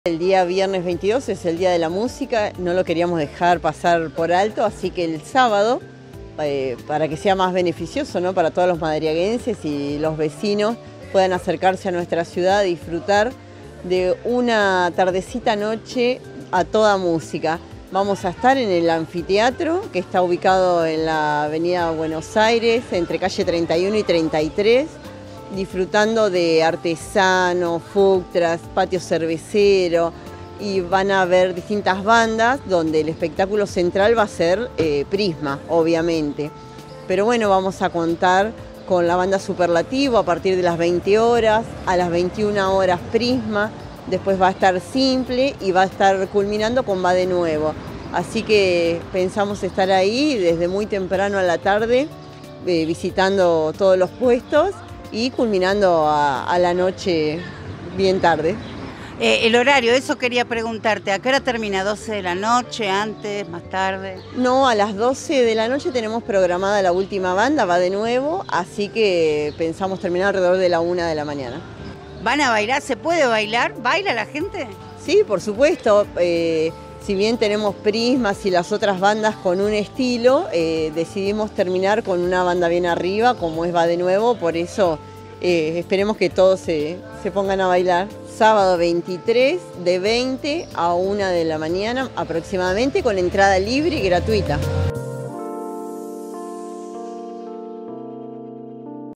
DÍA DE LA MÚSICA EN EL ANFITEATRO
Karina Uribe, Secretaria de Cultura, Turismo y Educación, invita a todos los habitantes de la región a celebrar el Día de la Música este sábado 23 de noviembre en el Anfiteatro del Paseo del Bicentenario. 🎉🌟

¡Ven a disfrutar de un evento lleno de música en vivo y buena compañía! 🎶💃🕺 No te lo pierdas, ¡será una fiesta para todos los sentidos! 🎸🎷